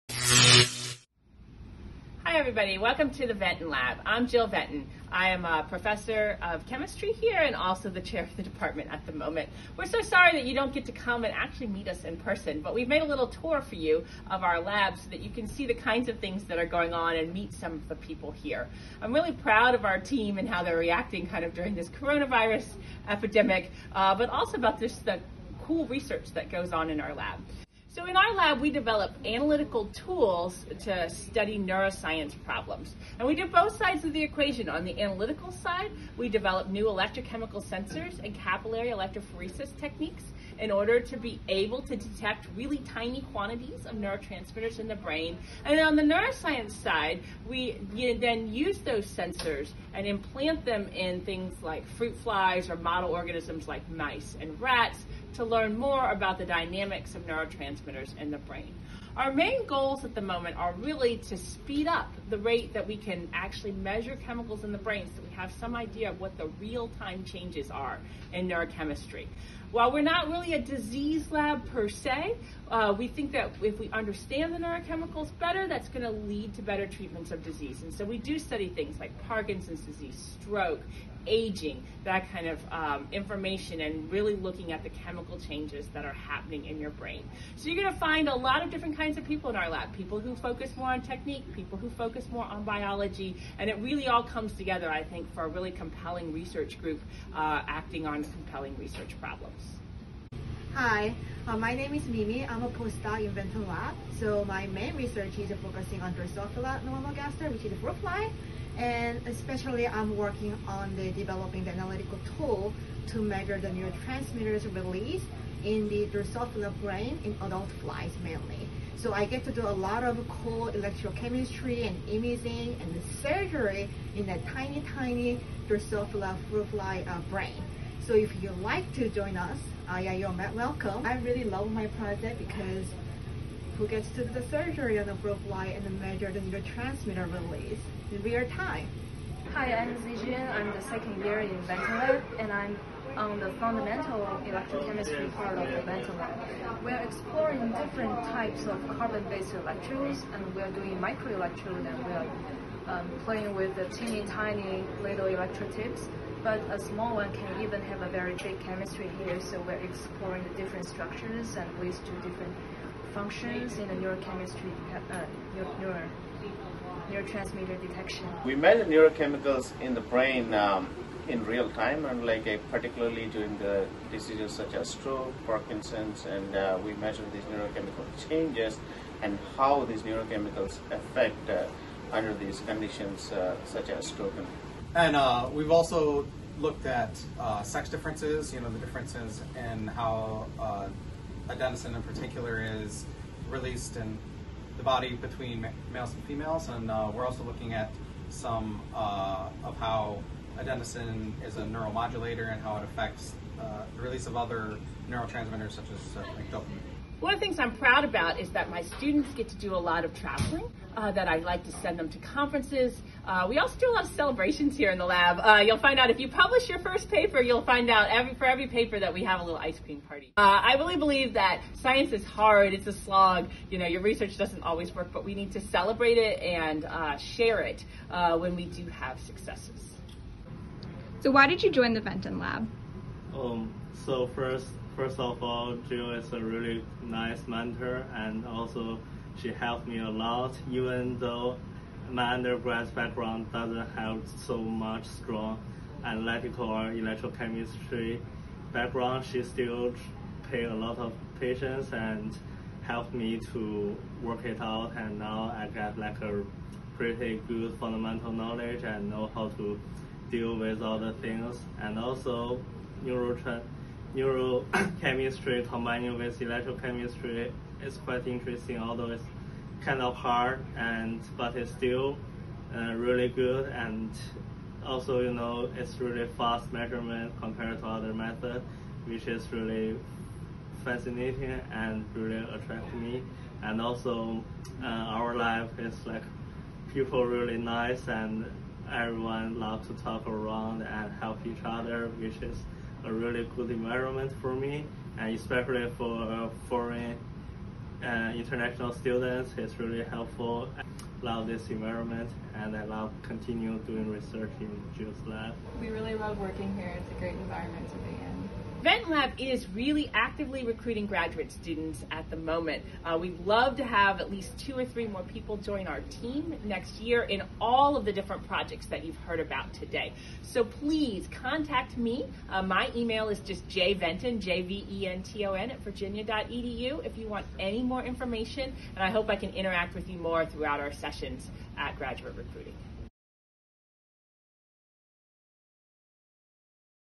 Venton Lab Video
Venton lab Graduate Recruitment Video 2020, University of Virginia Department of chemistry